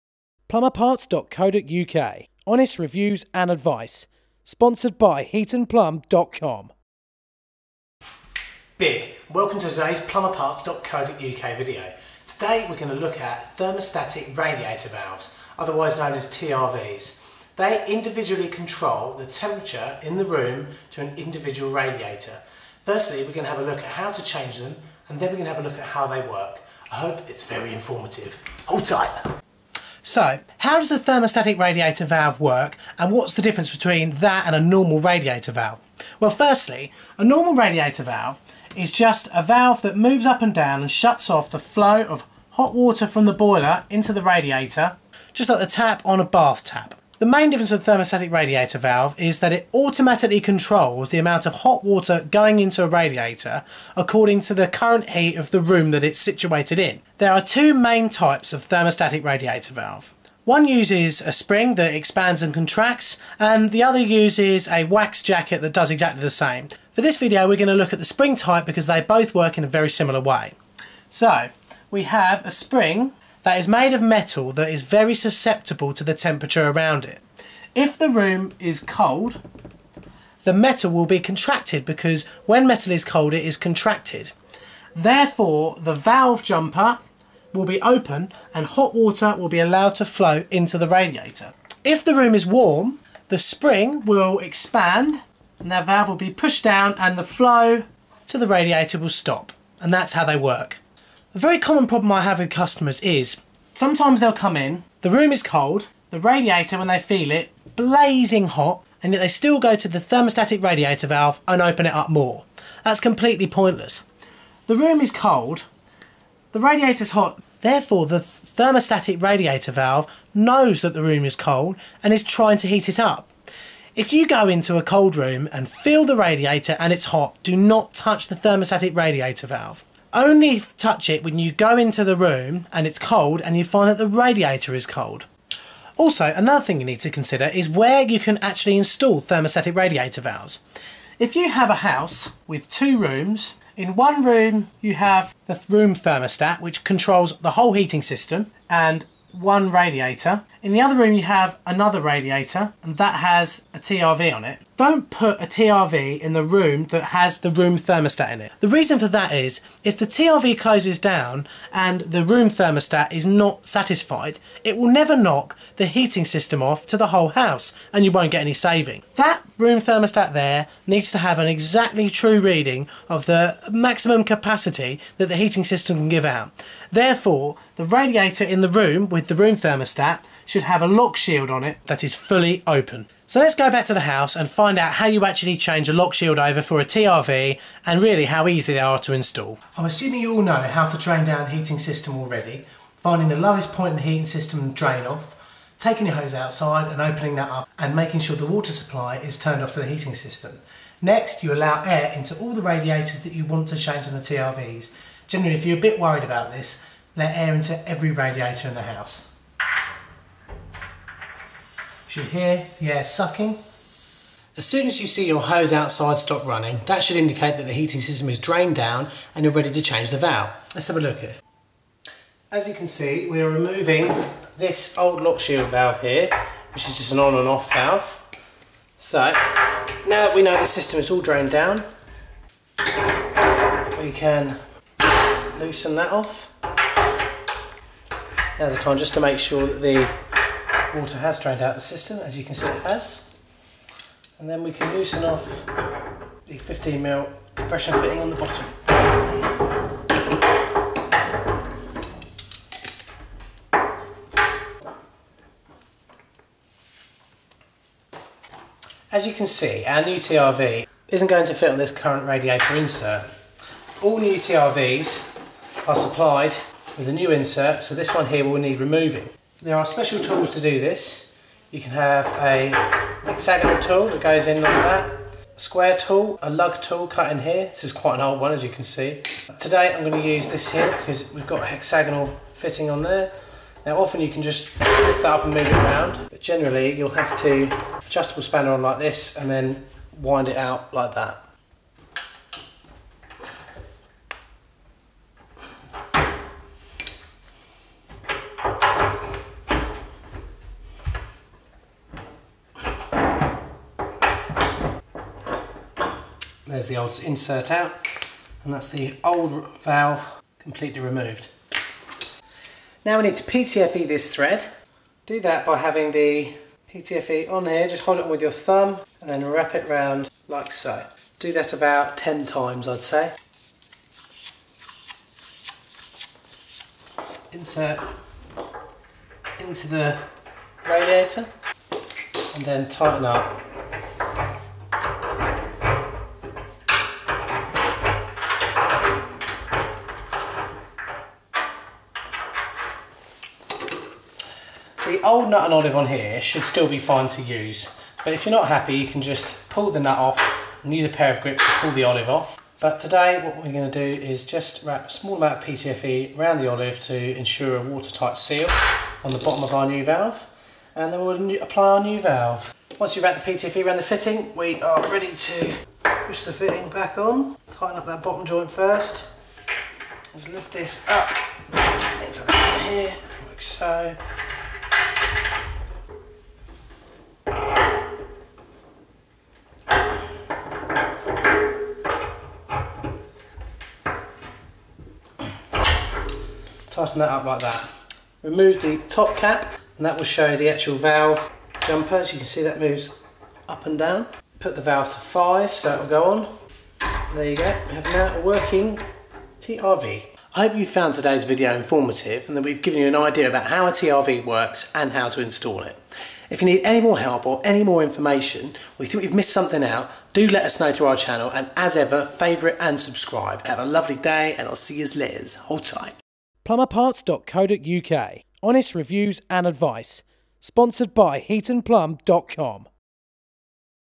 Thermostatic Radiator Valves - Plumbing Tips
This video covers thermostatic radiator valves. How they work and how you can install them. We also cover which room to install them and why.